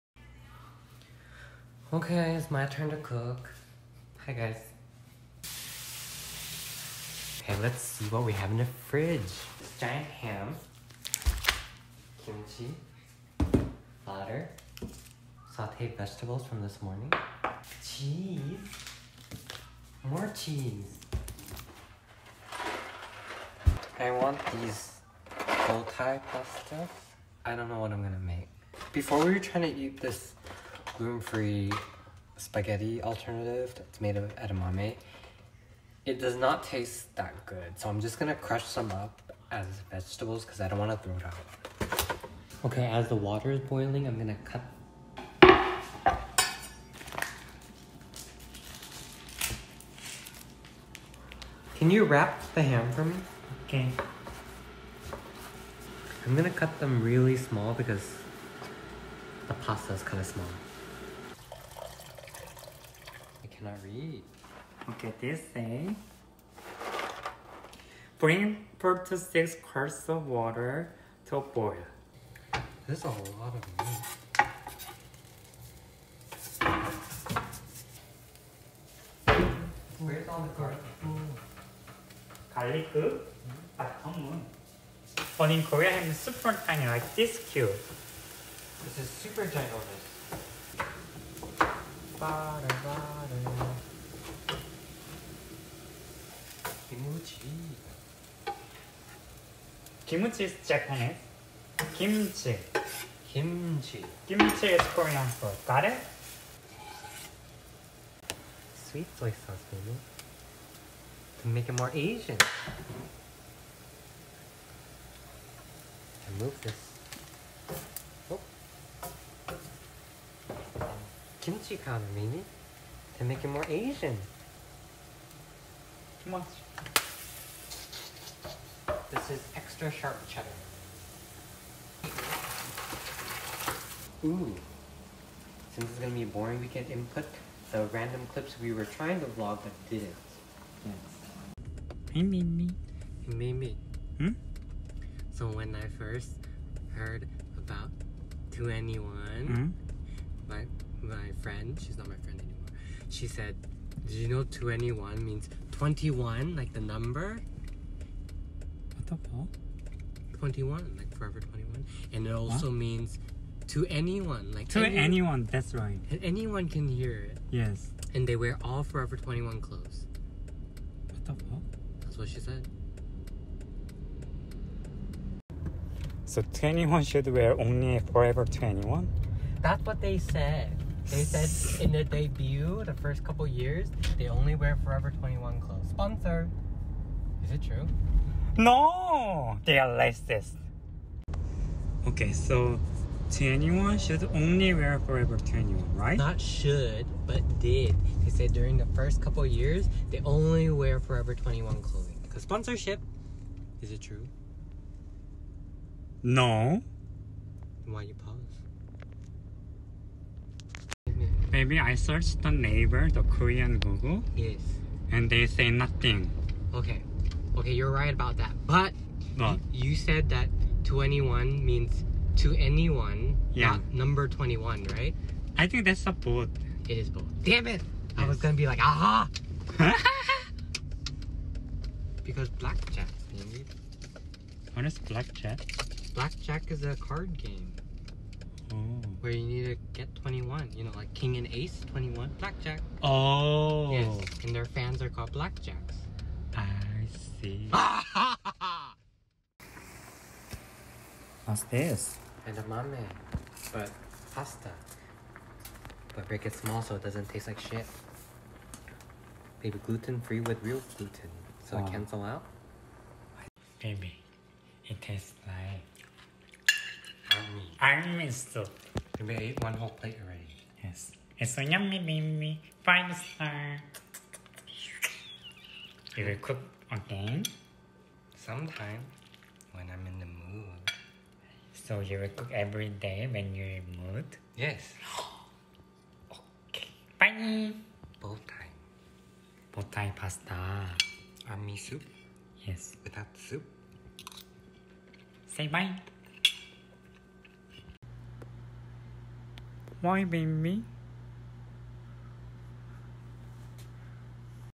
kimchi bowtie pasta with cutie gay korean husband
We make a weird pasta today!

And 2NE1 story! haha what you guys think?